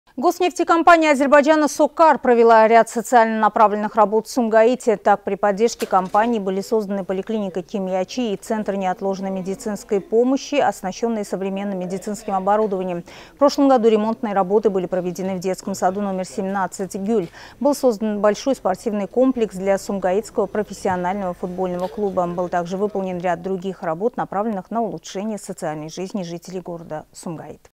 SOCAR реализует меры по улучшению социальной жизни жителей Сумгайыта
Госнефтекомпания Азербайджана (SOCAR) провела ряд социально направленных работ в Сумгайыте. Так, при поддержке компании были созданы поликлиника "Кимьячи" и Центр неотложной медицинской помощи, оснащенные современным медицинским оборудованием.
В прошлом году ремонтные работы были проведены в детском саду №17 "Гюль". Был создан большой спортивный комплекс для сумгайытского профессионального футбольного клуба. Был также выполнен ряд других работ, направленных на улучшение социальной жизни жителей города Сумгайыт.